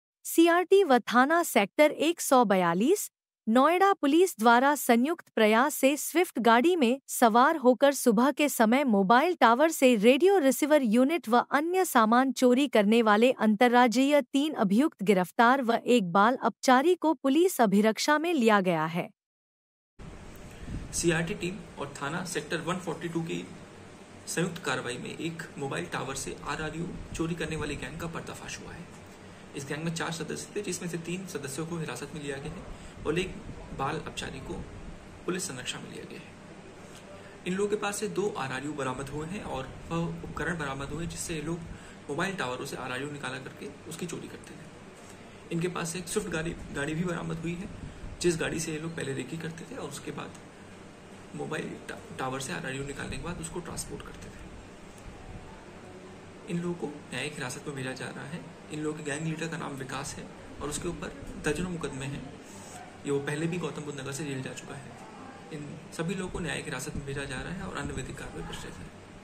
CRT व थाना सैक्टर 142 Noida Police द्वारा Mobile Tower से चोरी करने वाले 3 अभियुक्त गिरफ्तार
सीआरटी व थाना सैक्टर 142 नोएडा पुलिस द्वारा संयुक्त प्रयास से स्विफ्ट गाडी में सवार होकर रात्रि/सुबह के समय मोबाइल टावर से रेडियो रिसिवर यूनिट, बैटरी व अन्य सामान चोरी करने वाले अन्तर्राज्यीय 03 अभियुक्त गिरफ्तार व 01 बाल अपचारी को पुलिस अभिरक्षा में लिया गया है, कब्जे से 02 चोरी के रेडियो रिसिवर यूनिट मोबाइल टॉवर एंव घटना में प्रयुक्त 01 वायर कटर, 01 प्लास, 01 पेंचकस, 01 पाना/रिन्च, एक  स्विफ्ट कार।